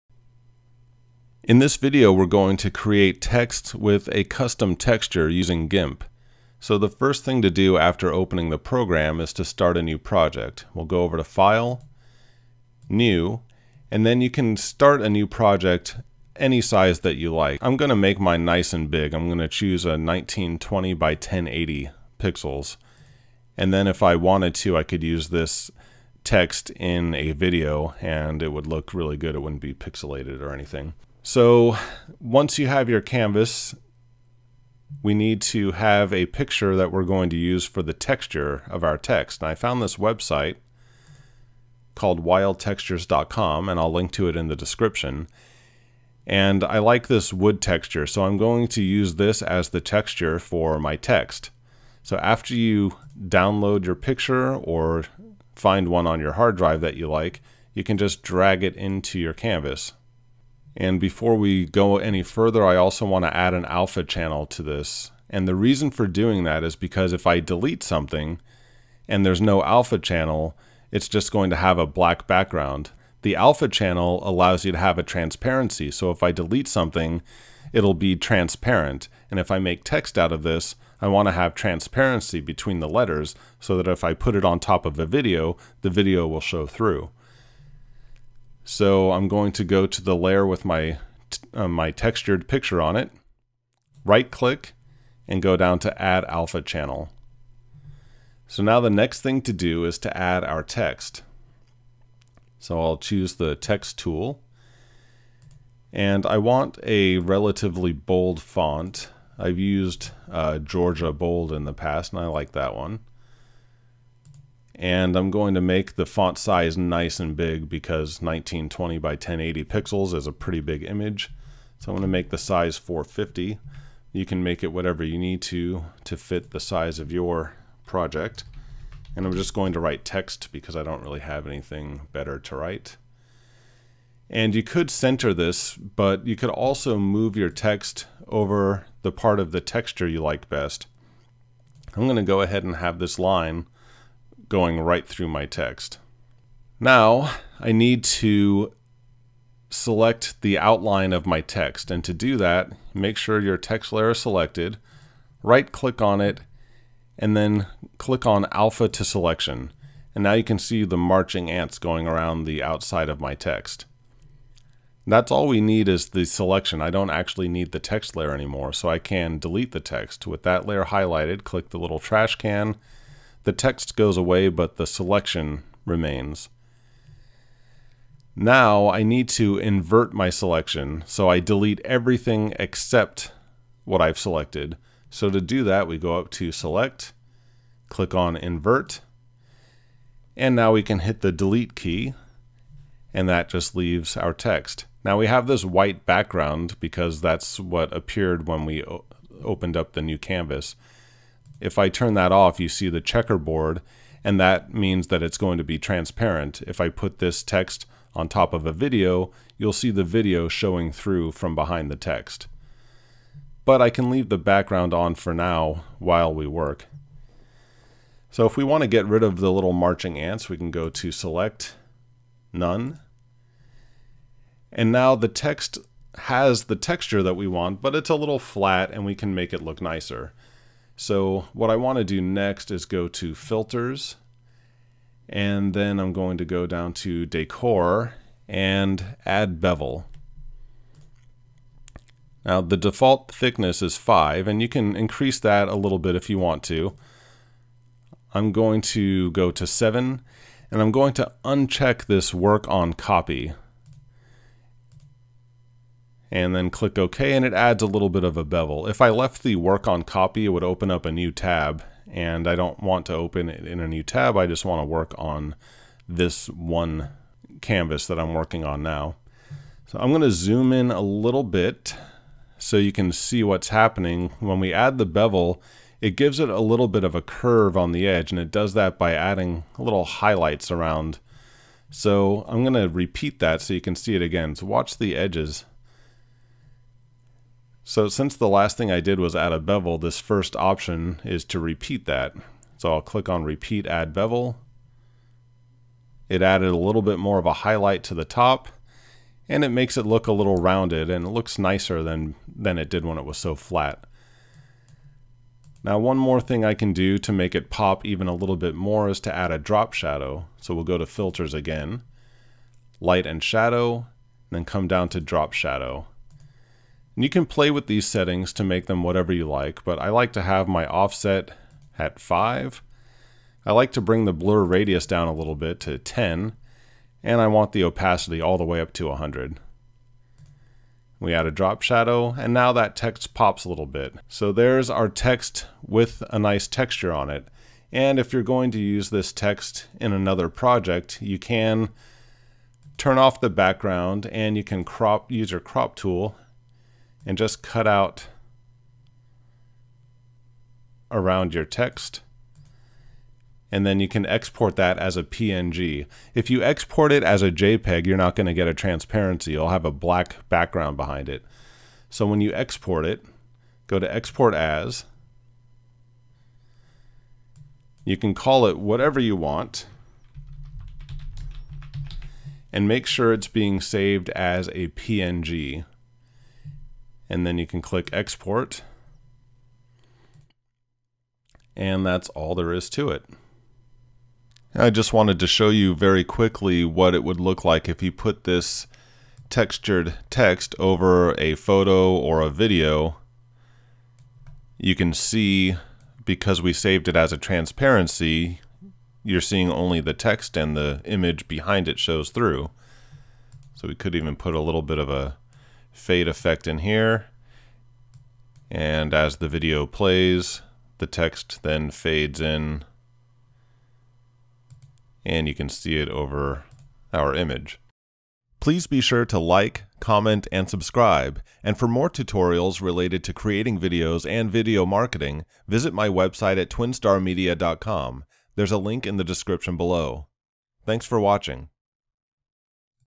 Create Text With a Custom Texture in Gimp Gimp Tutorial
I would also like to invite you to join my video creation and marketing Facebook Group

If you would like to follow along with this tutorial you can download Gimp

And you can use the same textured image I used by downloading it

Of course the texture you use this technique with can be any texture, you're not limited to using wood. You can use any texture that would best suit the project you're working on.

Also, although this video is about 7 minutes long, it doesn't take nearly that long to create this text effect. It takes longer to explain things as I go but you can create this effect in just a couple of minutes.